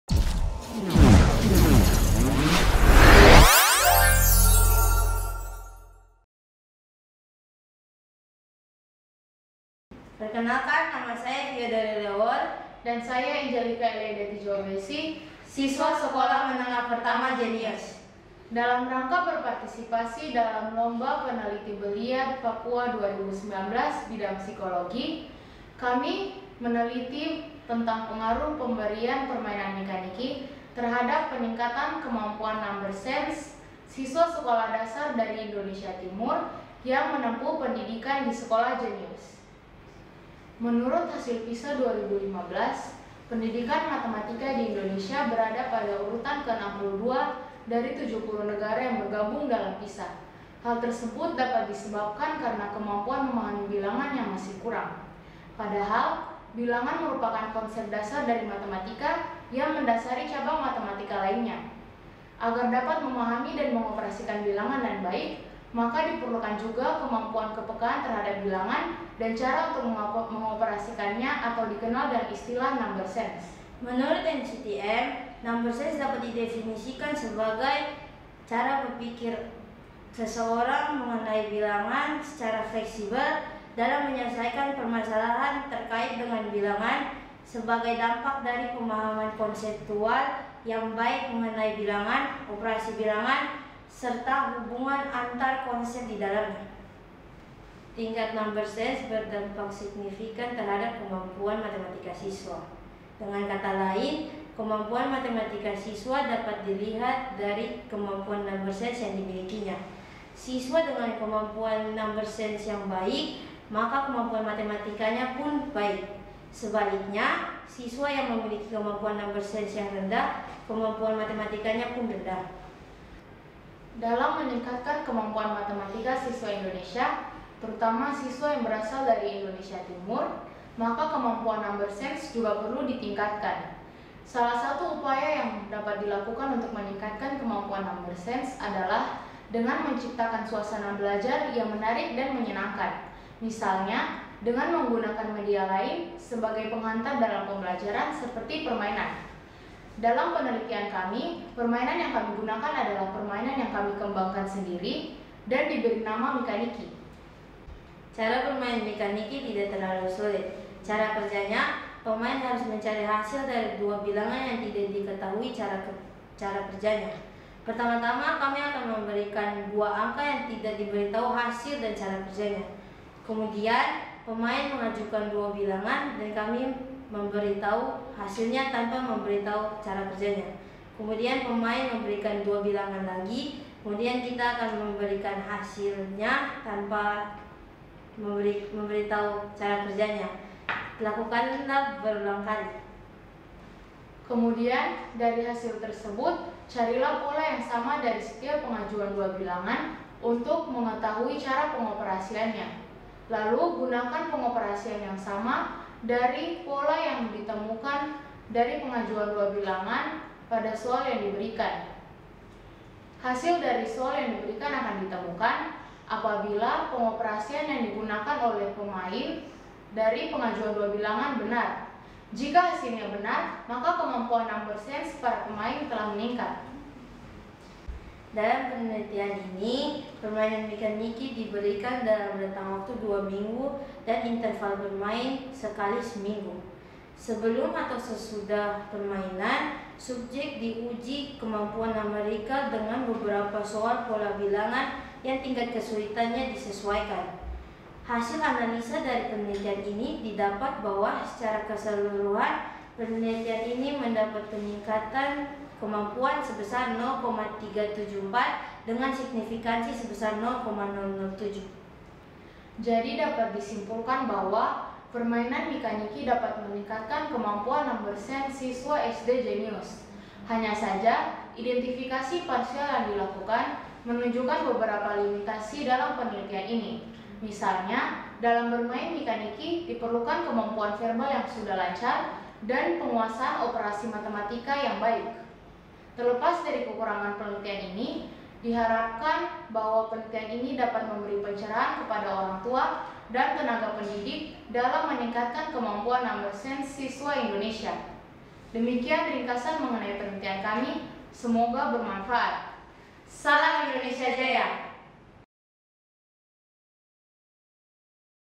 Math Game is A Game : How Mikaniki Helps Eastern Indonesia Students Improving Their Number Sense
Penelitian LPB 2019: Math Game is A Game : How Mikaniki Helps Eastern Indonesia Students Improving Their Number Sense (Fiodory Leuwol & Angelica Eleyda Hitijahubessy - Sekolah GenIUS).